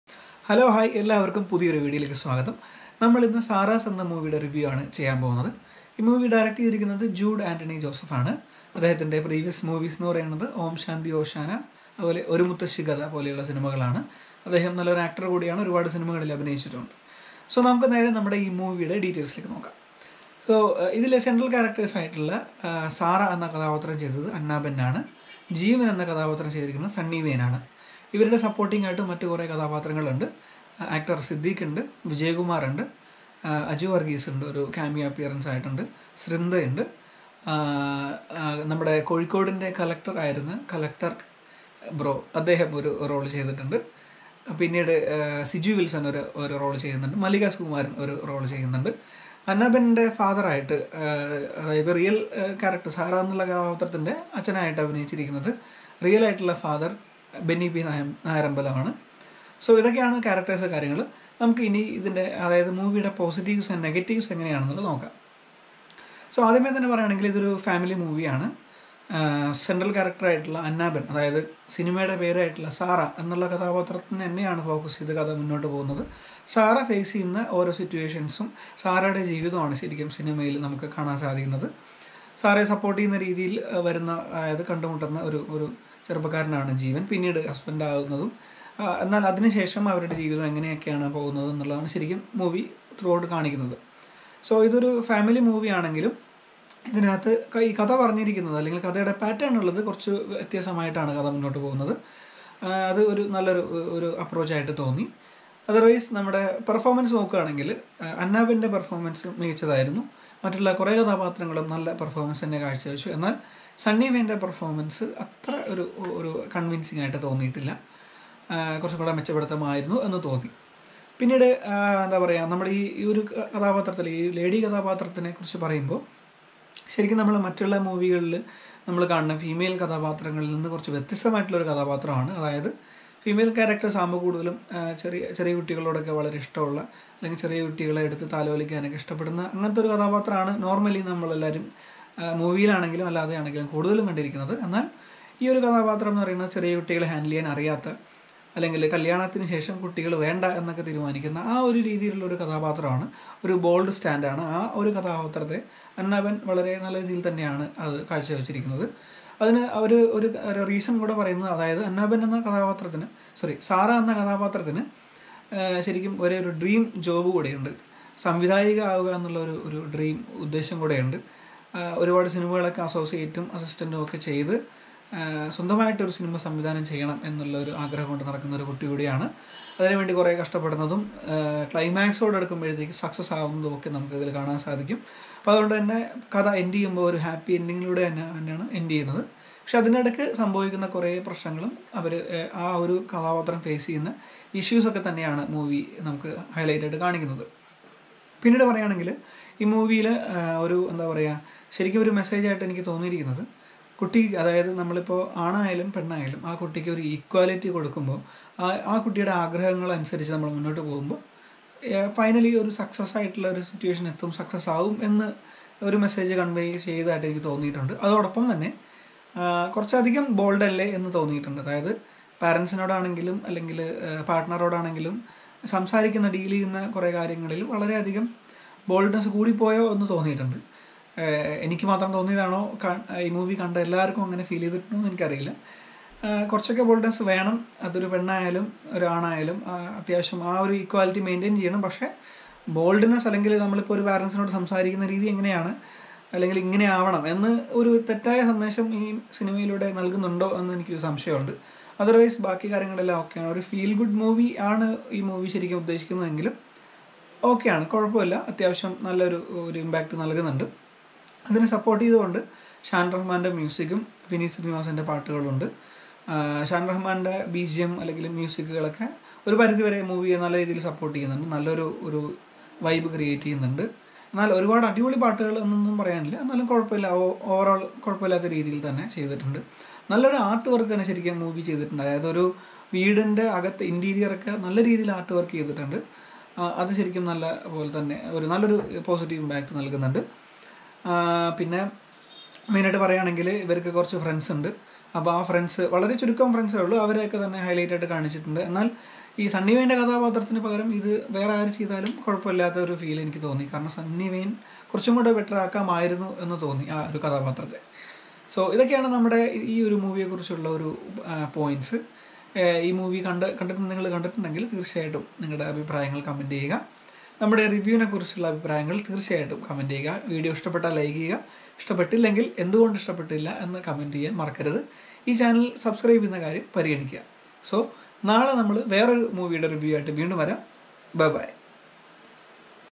SARA'S MOVIE REVIEW by ZiVlogger
Sara's Movie Plot

Sara Vincent is a girl in mid 20's who has no desire of giving birth to a child. Working as a co-director in the film industry, her biggest dream is to become a freelance director. She marries Jeevan Philip, a guy in his early 30's, who is in line with her vision of not having a child. The film then goes on to tell of the challenges and crises that befell Sarah in her life and work.